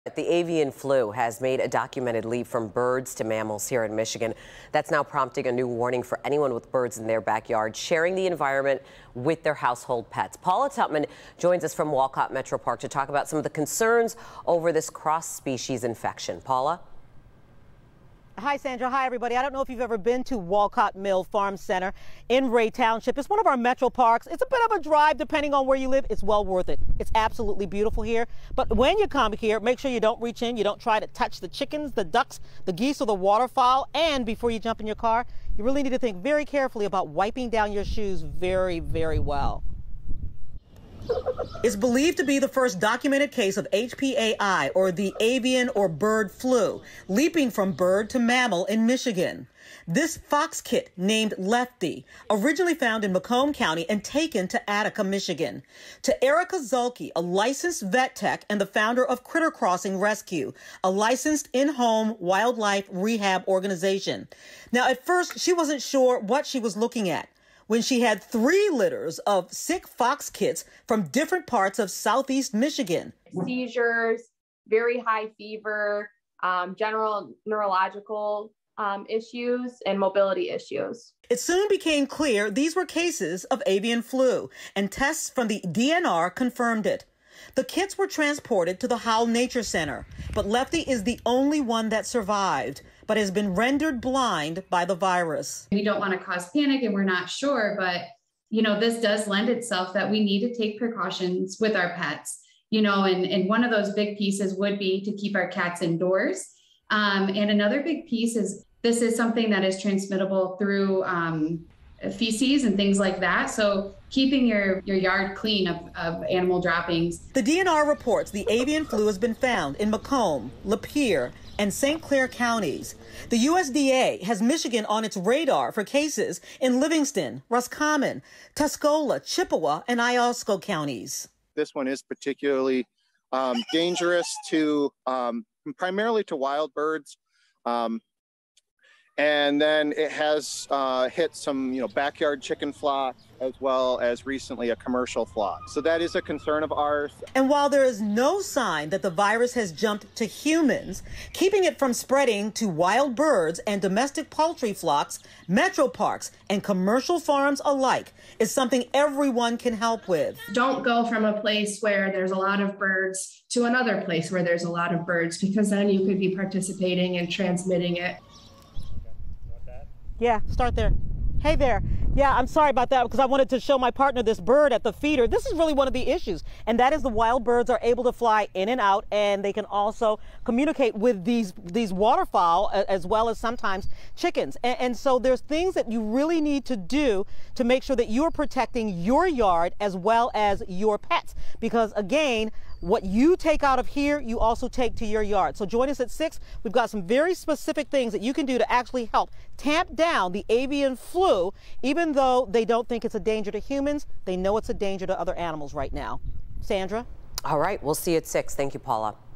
Bird Flu risk for animals and pets
The avian flu has made a documented leap from birds to mammals here in Michigan. That's prompting a warning for anyone with birds in their backyard, sharing the environment with their household pets.